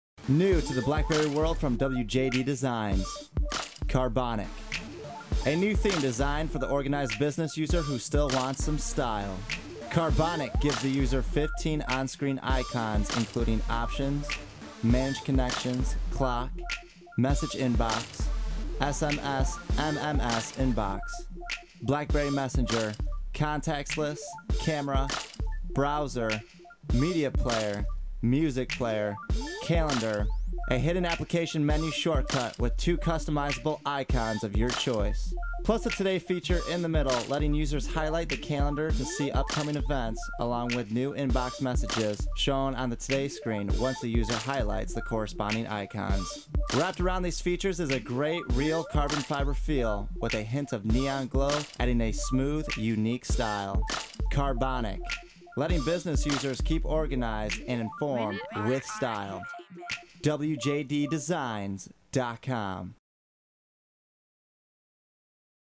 Carbonic :: Theme for the Blackberry Storm made by WJD Designs :: 15 On Screen Icons w/ Today
Welcome to Carbonic, the Father of my theme Azuric. This theme lets the business man or person on the go, who has no need for fancy hidden docks, get a full theme thats also organized. Boasting a 15 on screen Icon set (Options, Manage Connections, Clock, Messages, SMS/MMS, Messenger, Contacts, Camera, Browser, Media, Music, Calendar, Application Menu, and 2 custom Icons of your choice!) Even with everything on the screen, it has the today feature! Every icon is easy to touch and which makes Carbonic easily accessible. Through out the whole theme you see the style blend with the interface that we see with our stock storm. Go ahead try it out and let Carbonic be the theme you pick for your daily usage.

Features:

    * 15 On Screen Icons
    * 2 Customizable Icons
    * Today Feature
    * Easy to see and easy to touch
    * Fully Custom Interface
    * Every Screen is custom around the Stock Interface
    * WJD Designs Tag of Approval

Minimum Requirements:

    * Blackberry Storm 9500
    * OS 4.7
    * (Tested On 9530)

Display Dimensions
   480 x 360
Operating System
   4.7

-  Special Thanks to Caleb James for Voice Over